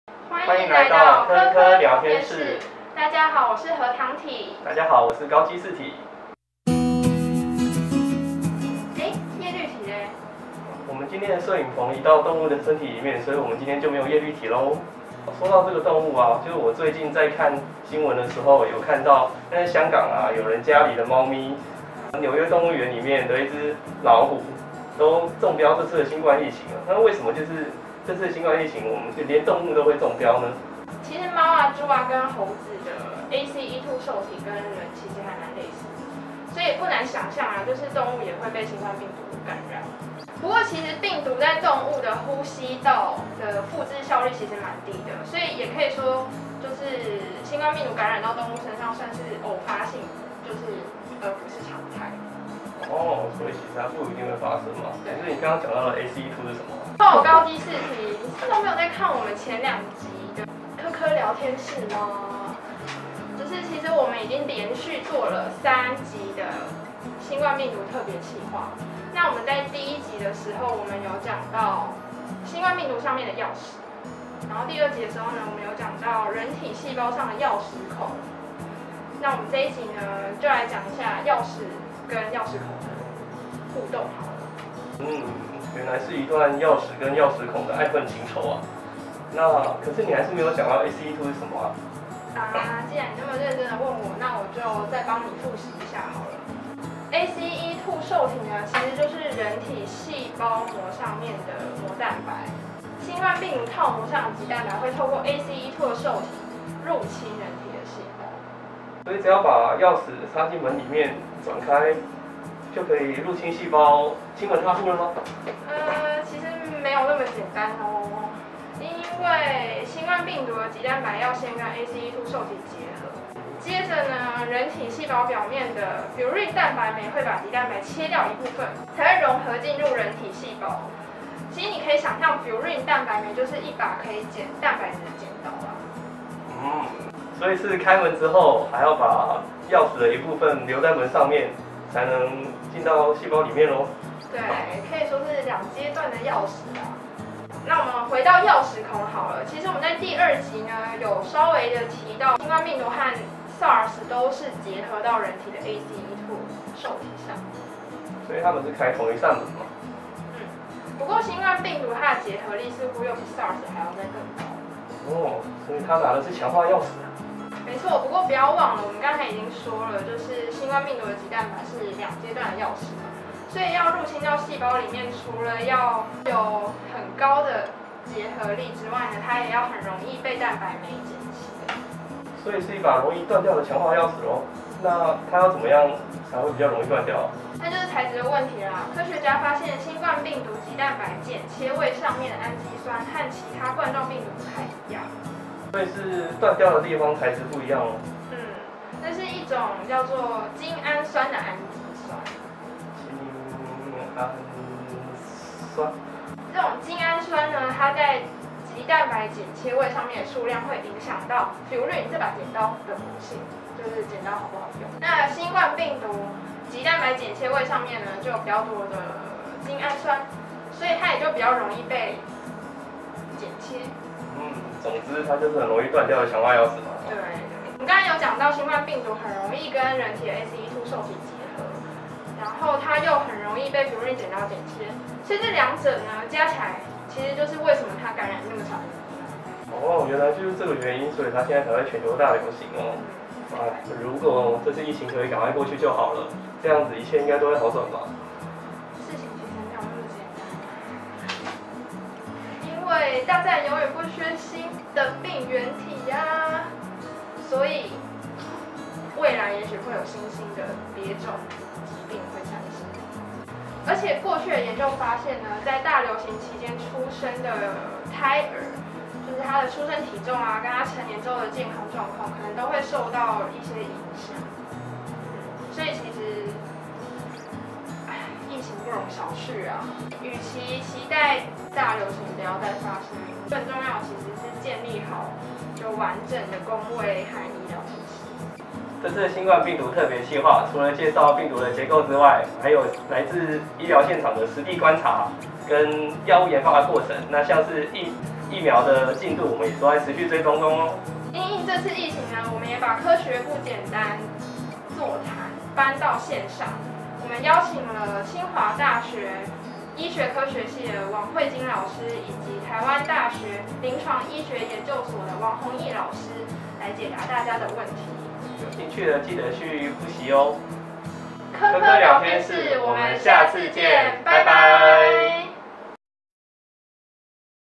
【科科聊天室 #19】透視新冠病毒！一段鎖孔與鑰匙的愛恨情仇！？
💉2020年5月號《透視新冠病毒》

穿隧式電子顯微鏡的鏡頭下，呈現一場人類與新冠病毒的戰役。至今雙方仍持續搏鬥，唯有知己知彼，才能早日贏得勝利。從分離純化、全面剖析，到開發藥物，科學家極力對抗這個難纏的敵人。